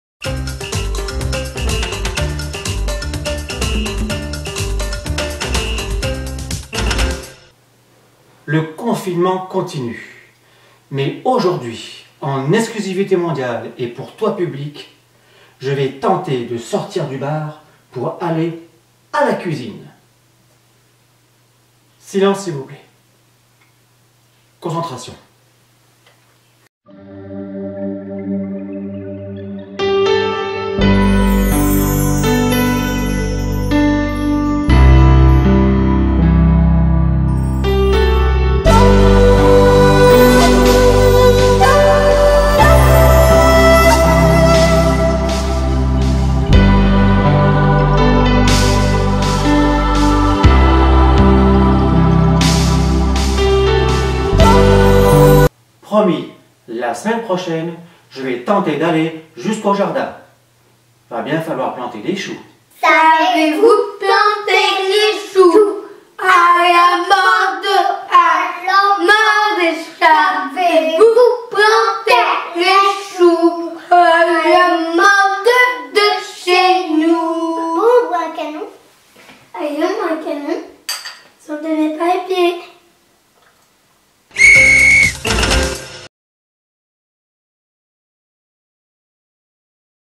Confinement en France : restezaubar, sortir du bar, une exclusivité mondiale......
16/04/20 : Une compilation de brèves de comptoir pour faire front au Coronavirus avec l'arme de l'humour.

Le confinement continue... mais restezaubar a décidé de sortir pour aller à la cuisine... une exclusivité mondiale...